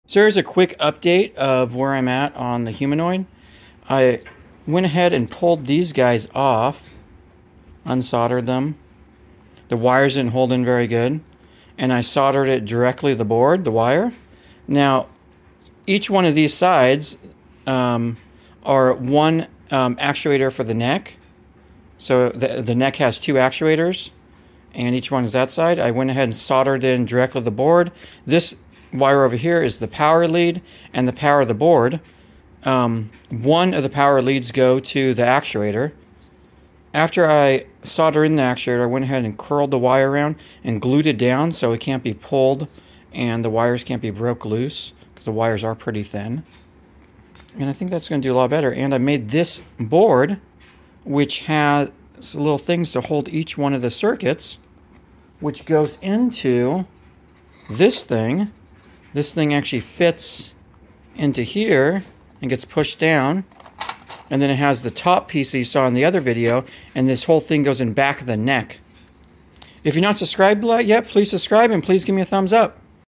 Upgrading Taylor's Neck Electronics: Soldering for Stability!
In this video, I dive into the electronics for Humanoid Taylor's neck assembly! I replace the temporary screw-in fittings on the driver board with a more secure soldered connection. Watch as I showcase the updated setup and explain how this improves reliability for Taylor's movements.